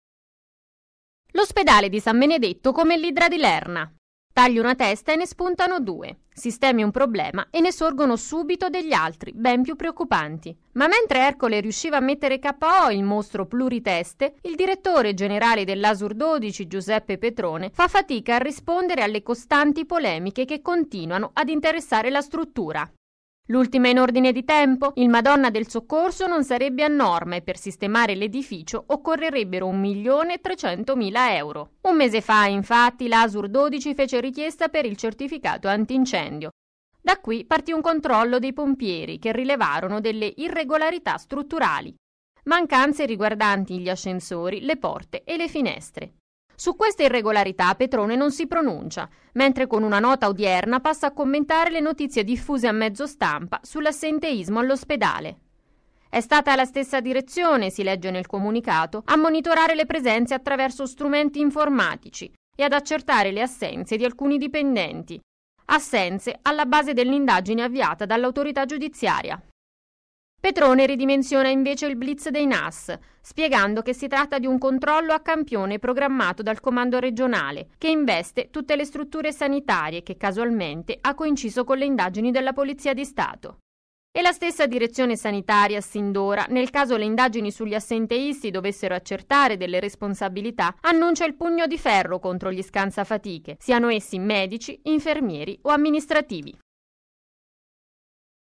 San Benedetto - Dopo i NAS e l'assenteismo si scopre che l'ospedale non è a norma
il madonna del soccorso continua ad essere nell'occhio del ciclone